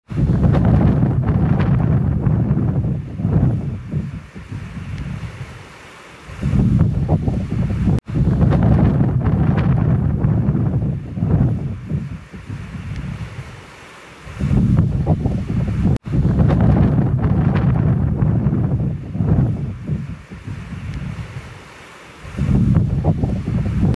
100% yama photo
or "video w beads ov light traveling up and down as reed rolls in soft waves like -o-- --o-- --o-"
text eye want somehow to append here is:
1 hitotsu seagul on mountain lake tonde 堂々巡り
Ippiki seagull what is your qualm